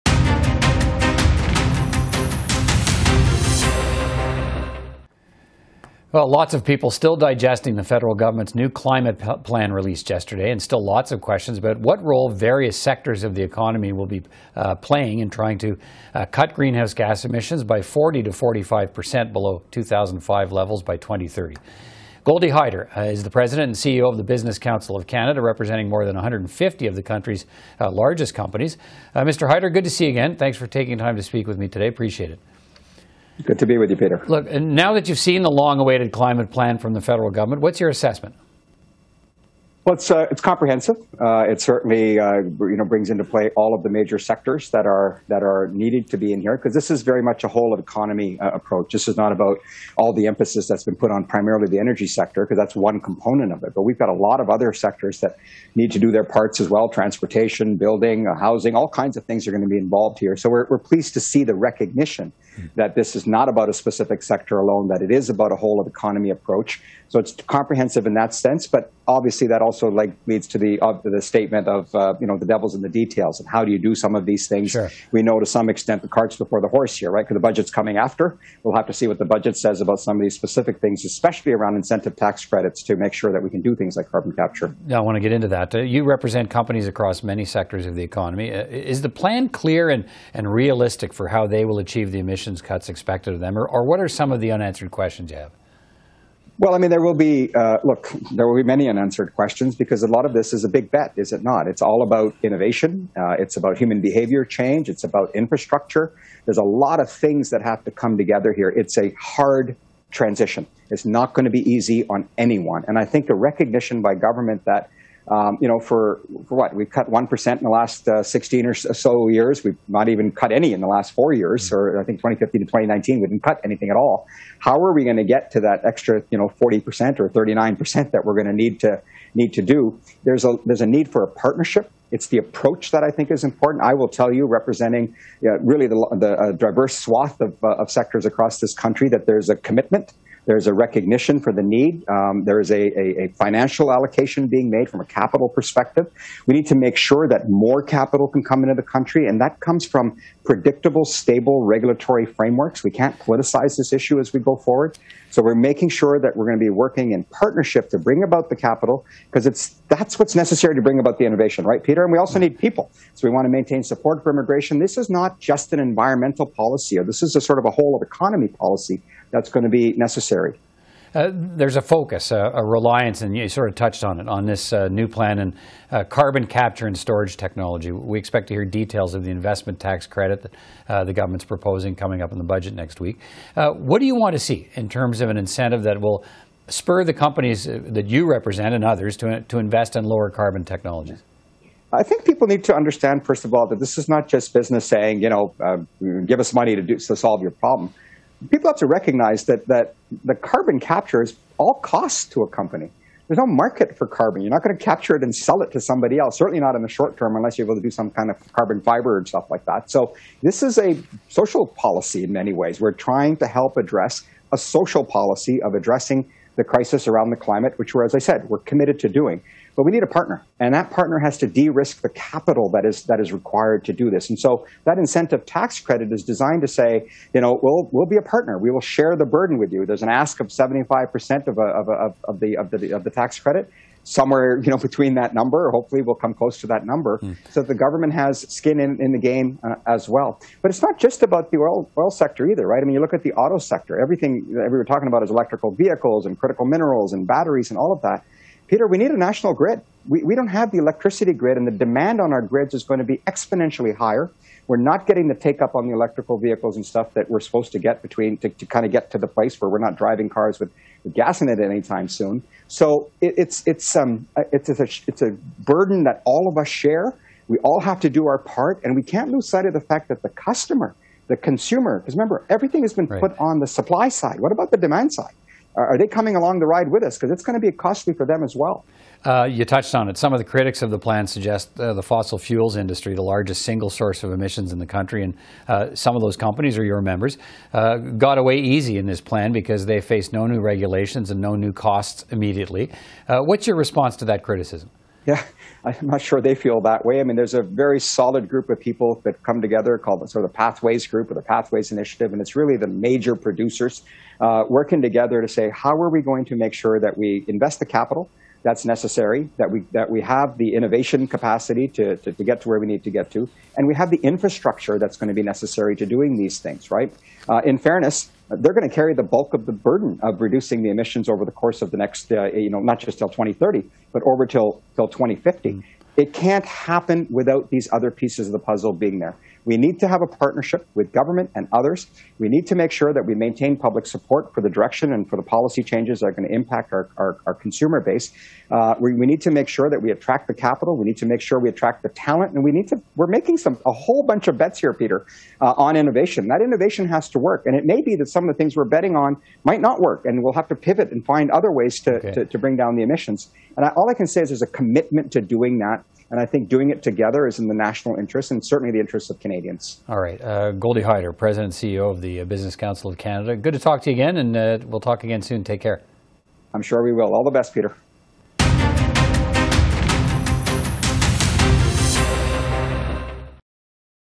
Business Council of Canada president reviews Canada’s climate plan and its impact on the economy
Goldy Hyder, president and CEO of the Business Council of Canada, reviews how the federal government’s new climate action plan will impact? the economy. He also touches on his predictions for this year's federal budget, which is set to be unveiled on April 7.